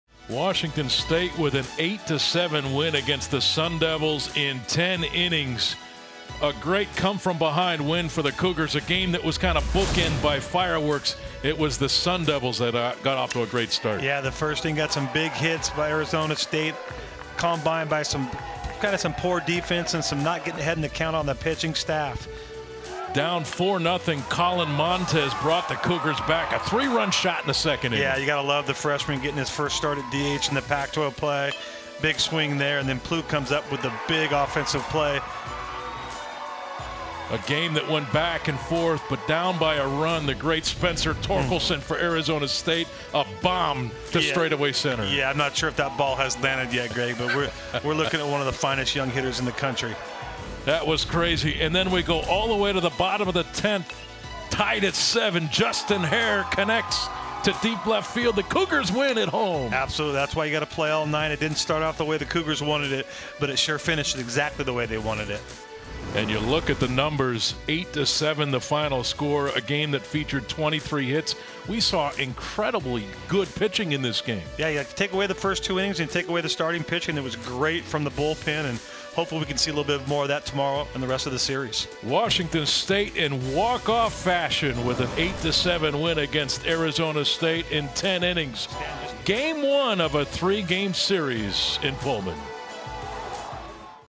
Recap: Washington State baseball walks off against Arizona State behind Justin Harrer's home run
Pac-12 Networks' Greg Heister and Donnie Marbut break down Washington State's 8-7 win over ASU on Thursday in Pullman. Justin Harrer connected in the 10th inning with a walk-off solo homer.